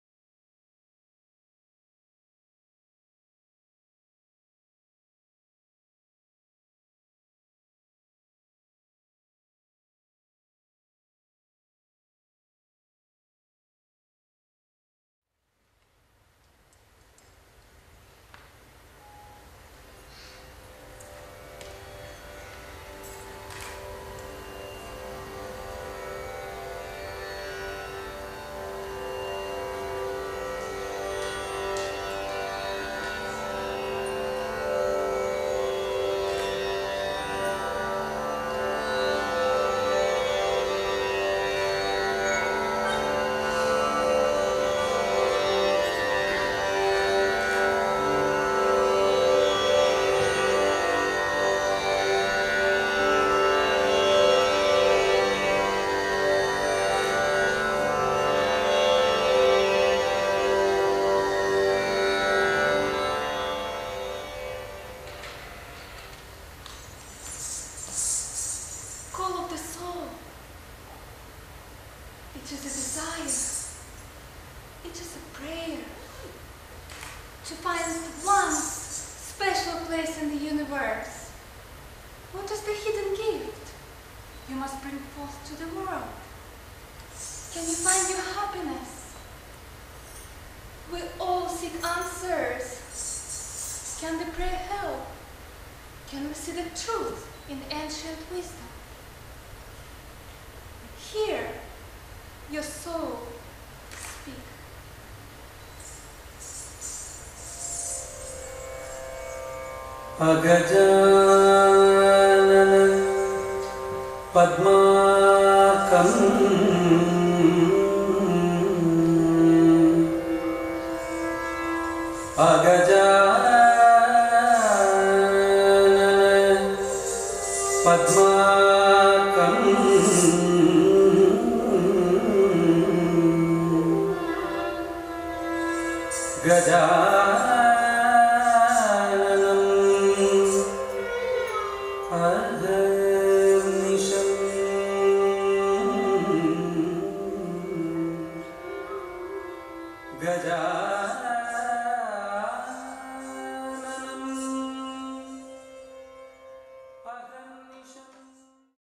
Oxana Banshikova ( Call Of The Soul )
Indian Classical Dance recital by Oxana Banshikova

Call Of The Soul

It is a desire, it is a prayer, to find one's unique place in the universe. What is the hidden gift you must bring forth to the world? Can you find your happiness?
Can you hear your soul speak?
Using traditional repertoire and mesmerizing sculpturesque technique of BharataNatyam Indian Classical dance, Oxana will explore how longing for a union of Jeevatma (individual soul) and Paramatma (universal soul) represents one's search for happiness.

The simple truth is, at the end of everyone's journey, they are one and the same.

Video by OKENVIDEO